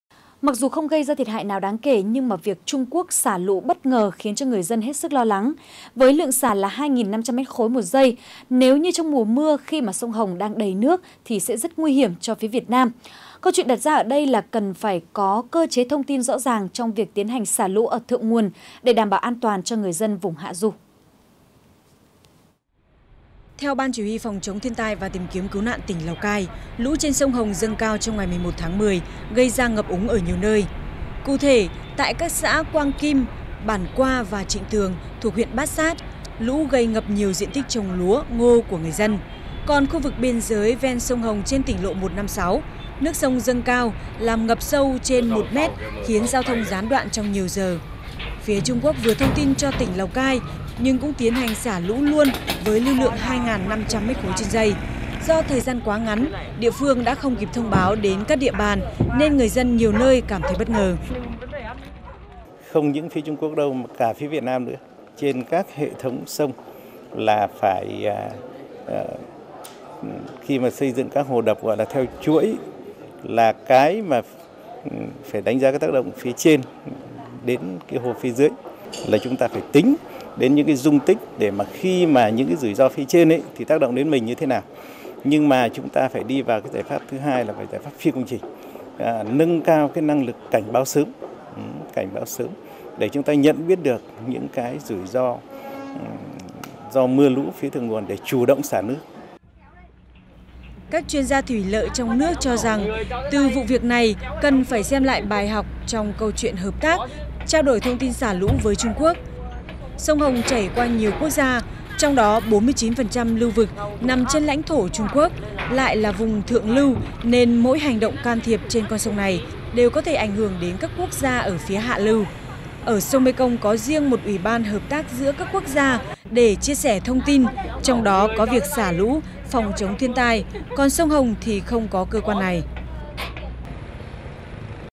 VTC14_Cần cơ chế ràng buộc trong việc Trung Quốc xả lũ
(Truyền hình VTC14) - Việc Trung Quốc xả lũ bất ngờ khiến người dân hết sức lo lắng. Với lượng xả 2.500 m3/giây, nếu trong mùa mưa, khi sông Hồng đang đầy nước sẽ rất nguy hiểm cho phía Việt Nam.